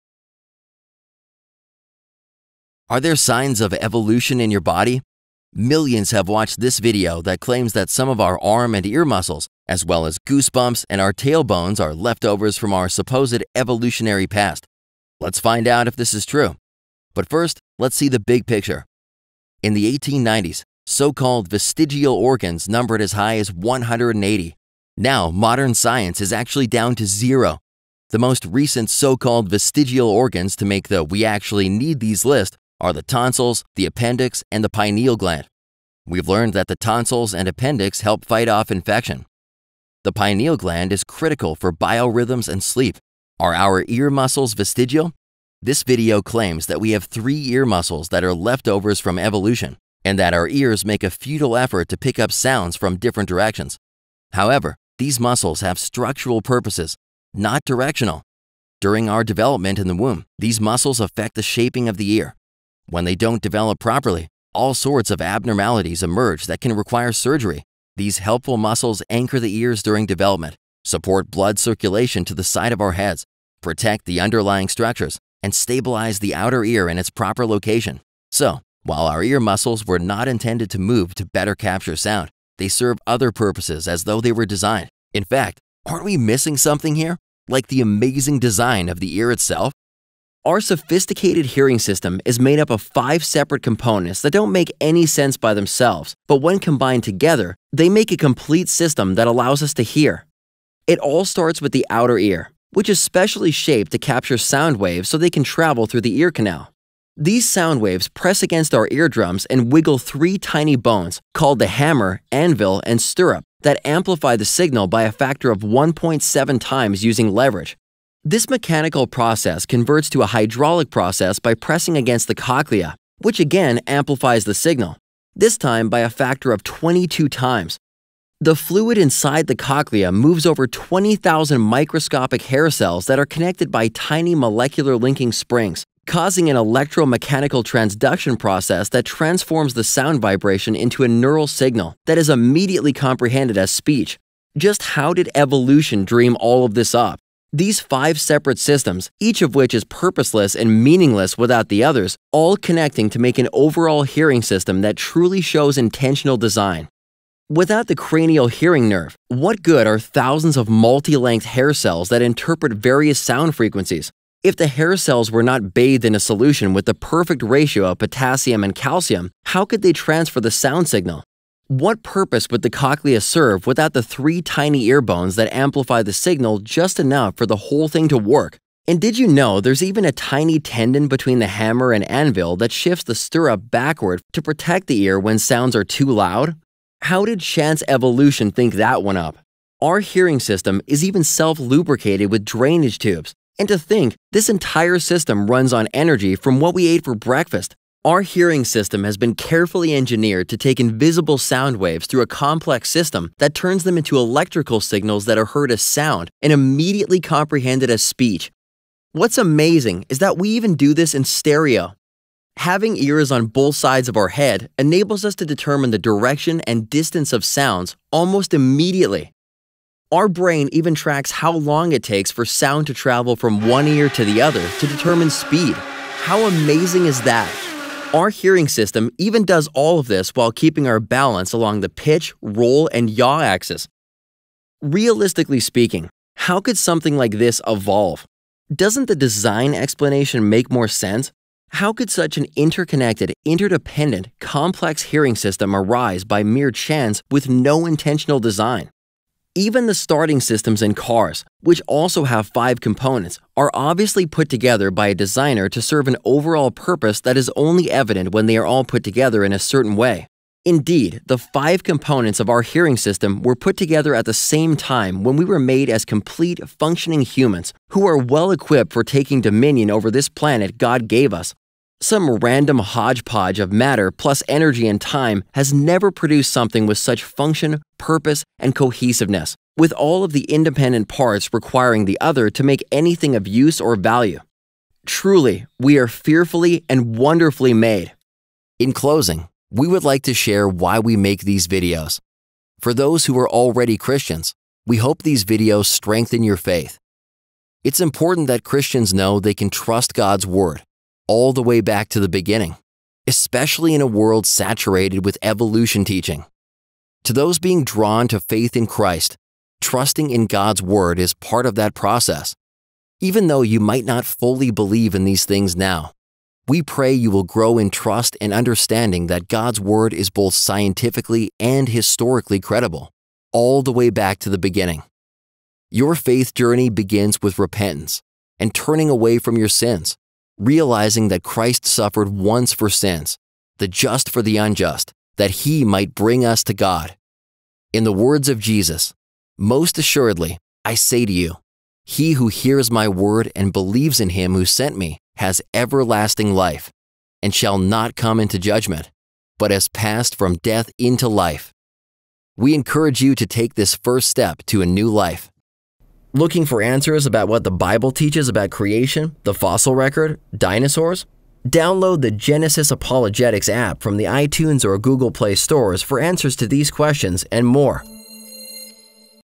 Are There Signs of Evolution in Our Body? Part 2- Ear Muscles and the Hearing System
Are there signs of evolution in our body? This video is Part 2 in our series that addresses the leading YouTube video on that same topic. This video addresses the ear muscles, including all three extrinsic auricular muscles—the posterior, superior, and anterior auricular muscles, and well as hearing in general. The next videos will address the other so-called "vestigial" parts of our bodies, such as the tailbone and goosebumps.